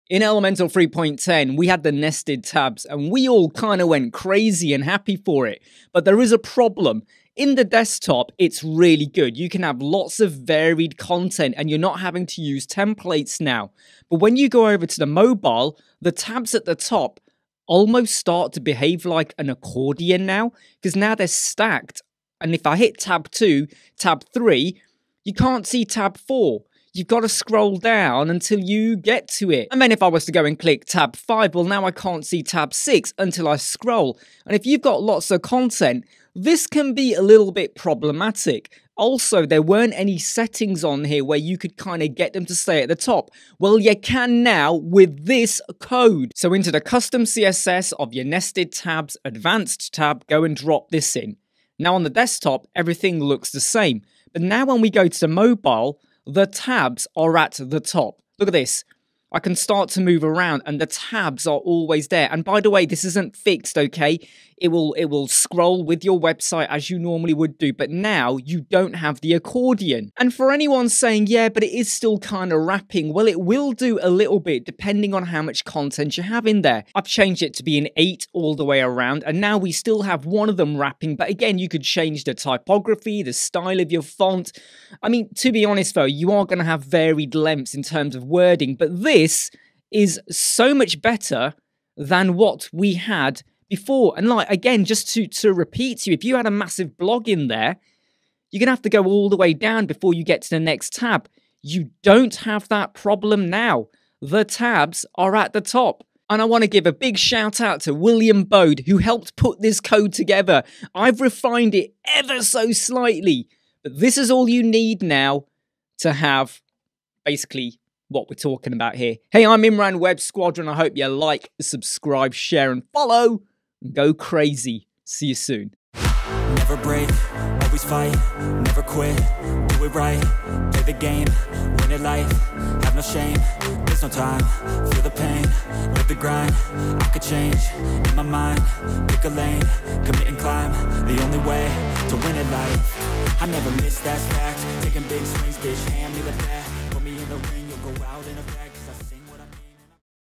Keep the Mobile Tabs for Nested Tabs at the Top - Elementor Wordpress Tutorial - Solution with CSS
This is how to keep the Mobile Tabs for Nested Tabs at the Top - Elementor Wordpress Tutorial - Solution with CSS that is so easy to do.

Code started by William Bode :)

CSS - add to the advanced tab of the Nested Tabs feature introduced since Elementor 3.10.